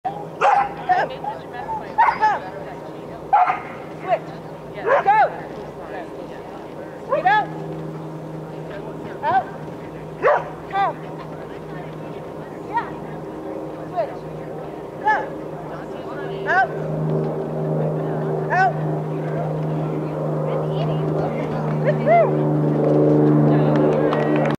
Willow Tunnelers R2 8 18 19
Willow, farm collie at 12 years old, completes a NADAC tunnelers course.